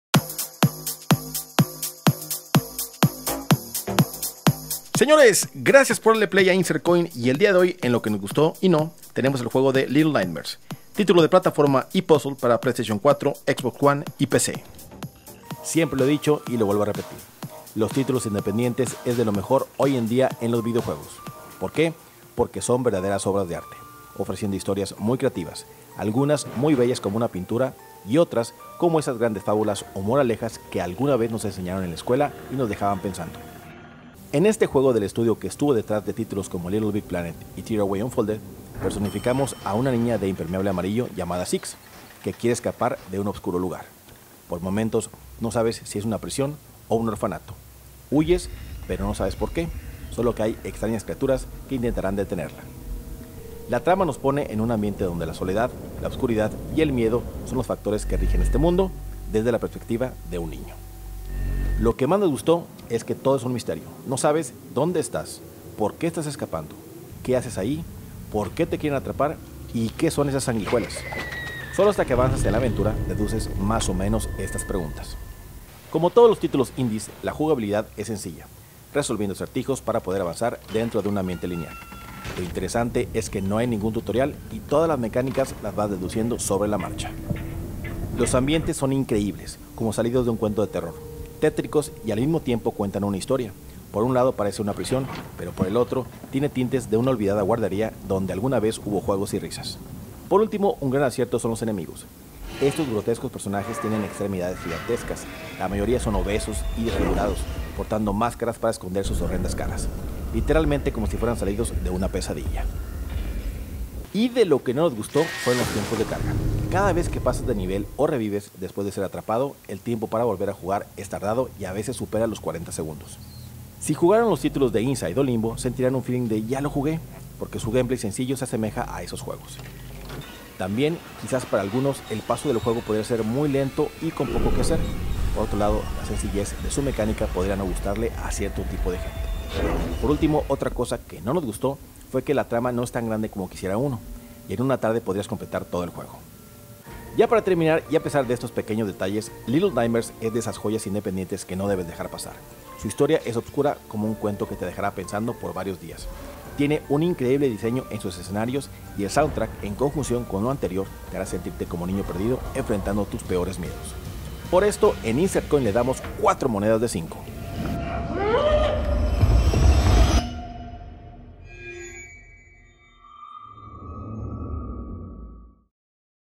LO QUE NOS GUSTÓ Y NO DE : LITTLE NIGHTMAERES
Little Nightmares, es un juego de plataforma, tipo independiente desarrollado por Tarsier Studios, quienes hicieron Tearaway Unfolded.

Personificamos a una niña de impermiable amarillo llamda “Six” que quiere escapar de un oscuro lugar.